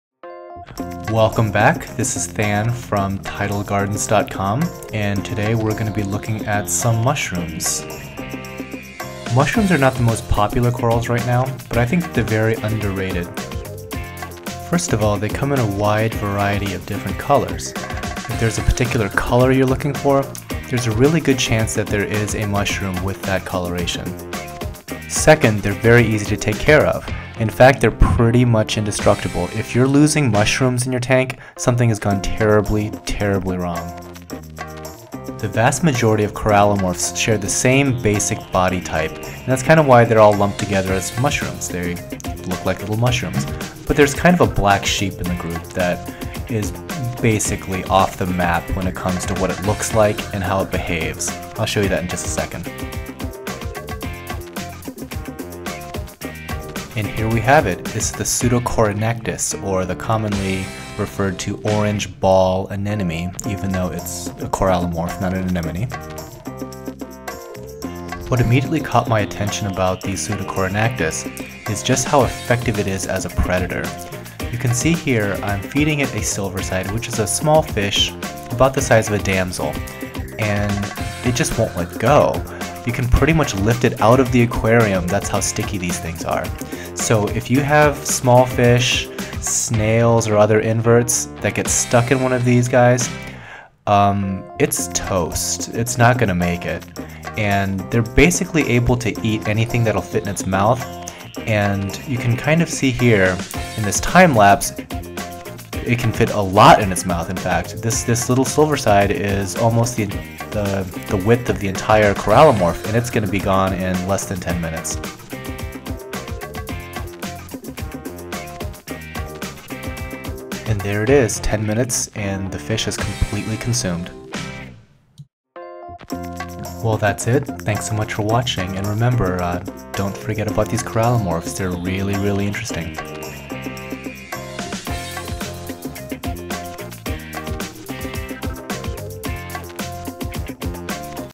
Mushroom Corals
Overview of different corallimorph mushrooms such as Rhodactis, Ricordea, and Discosoma with a more in-depth look at the Pseudocorynactis.